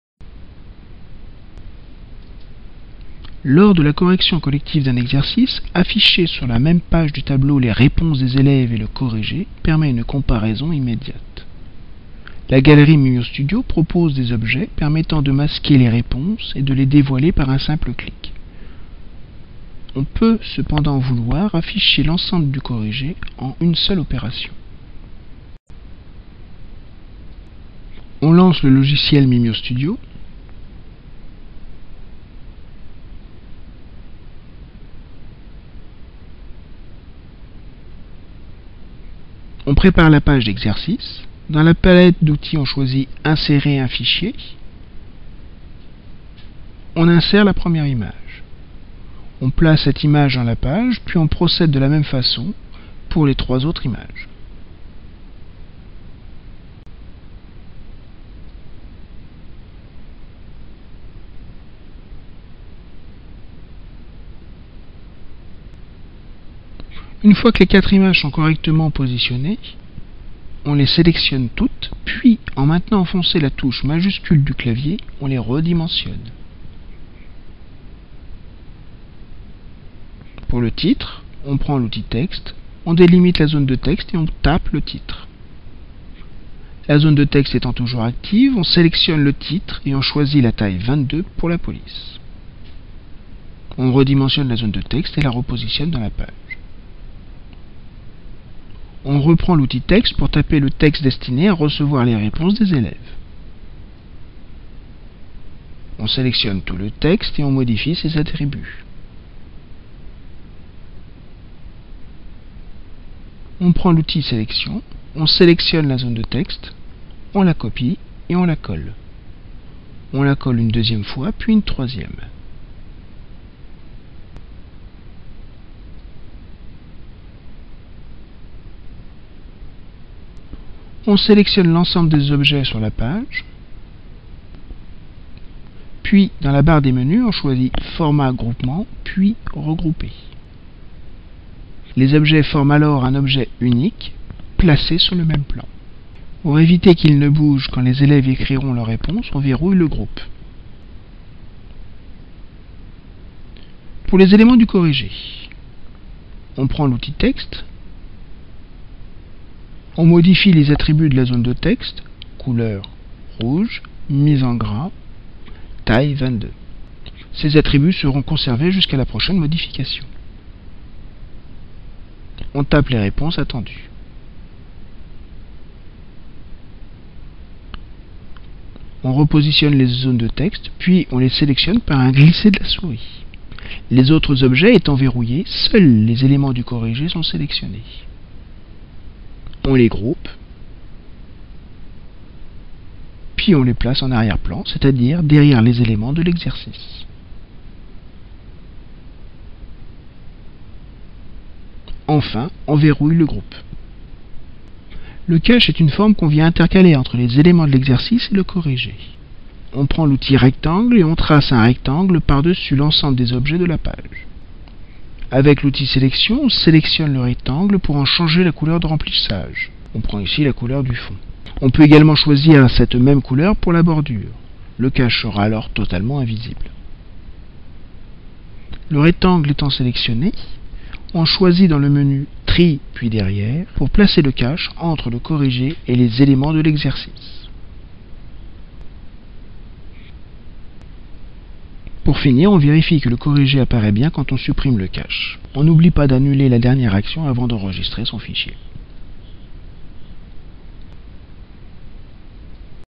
Afficher un corrigé - Bloc-Notes MimioStudio (Version 1)
Astuce avec l'outil Remplissage objet pour faire apparaître un corrigé, masqué au préalable lors de la construction d'un exercice.